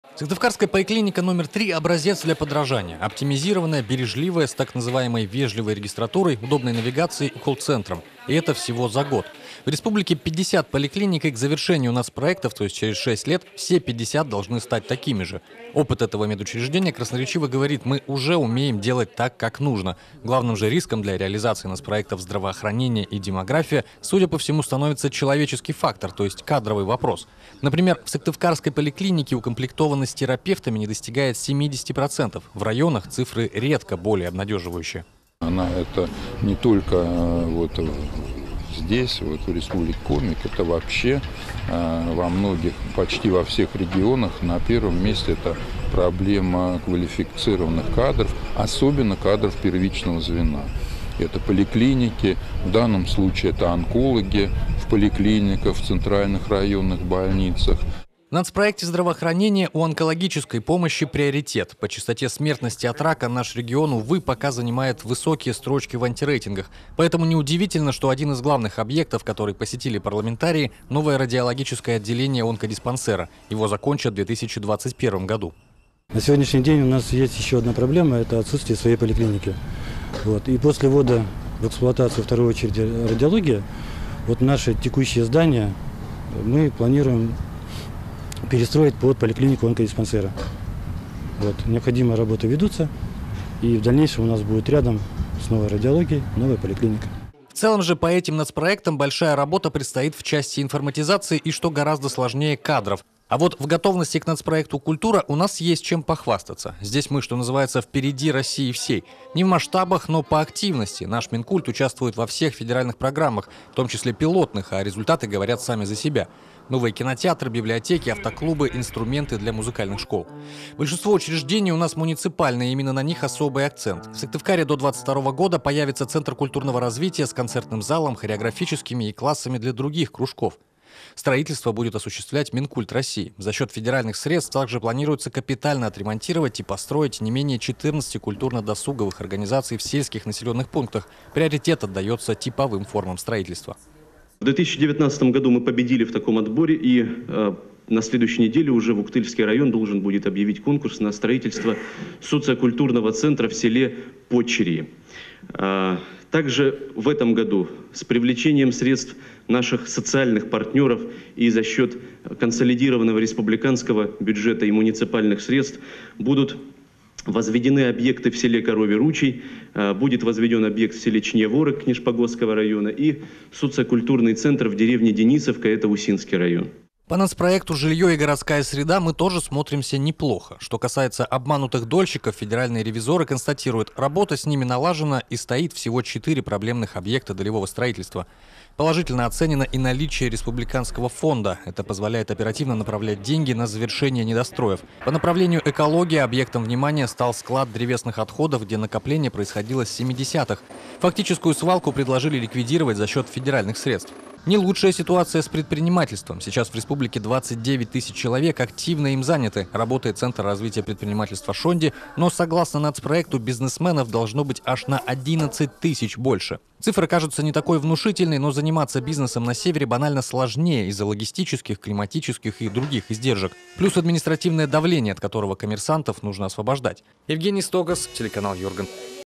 Нацпроекты в деле
Республика Коми в целом готова к реализации большинства национальных проектов.